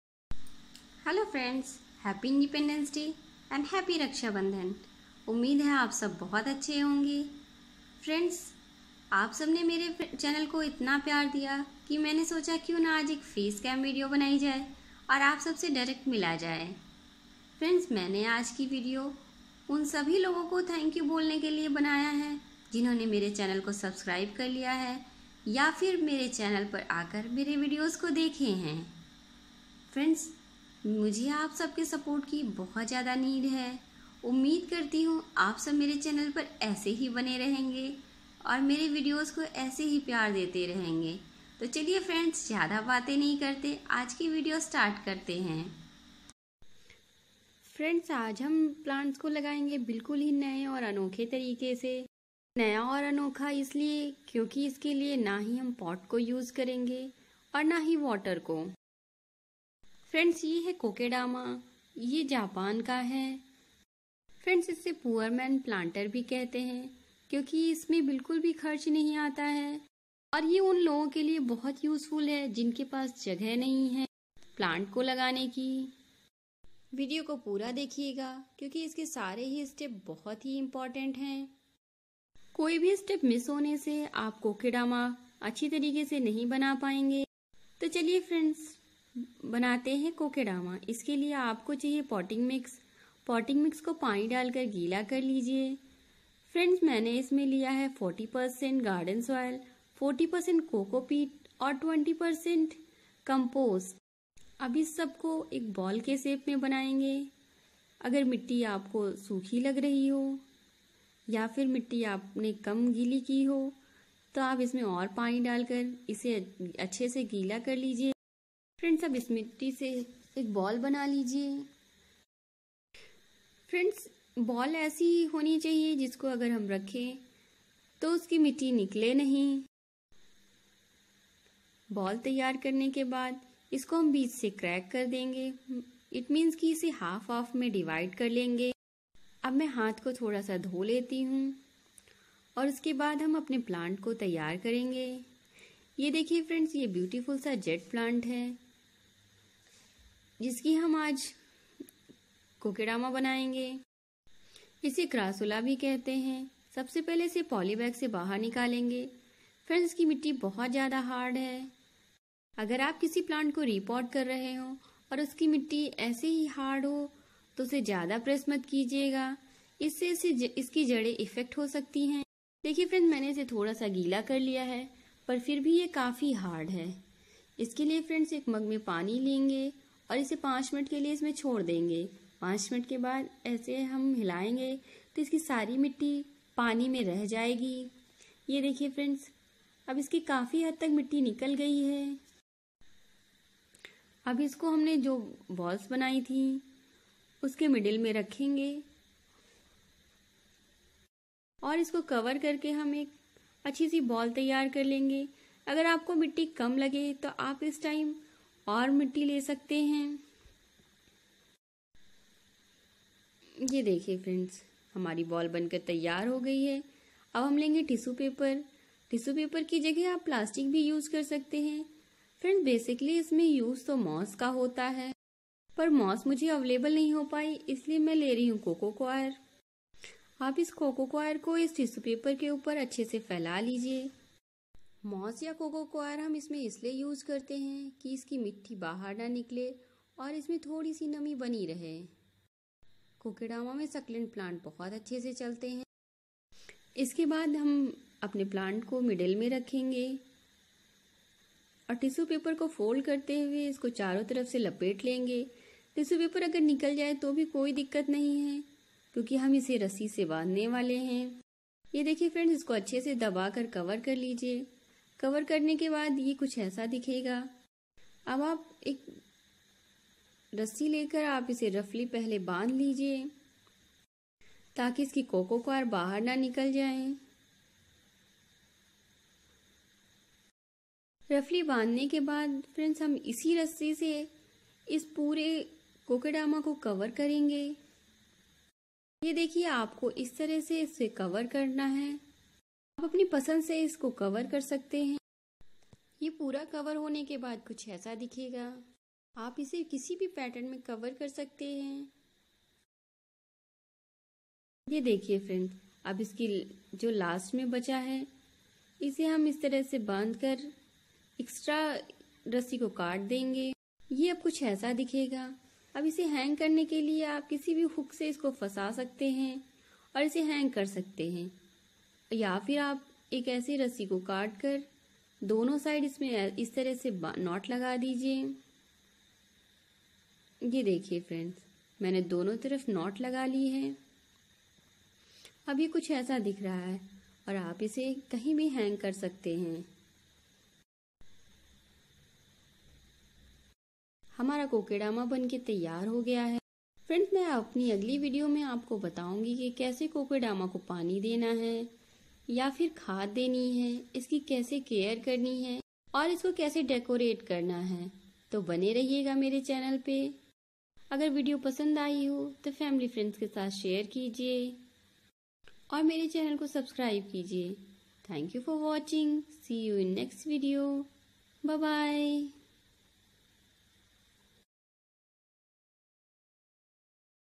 कैसे एक हैंगिंग kokedama बनायें - How to make Kokedama with Creative Gardening & DIY
Kokedama#making#easyway#home#string#love😋😋creative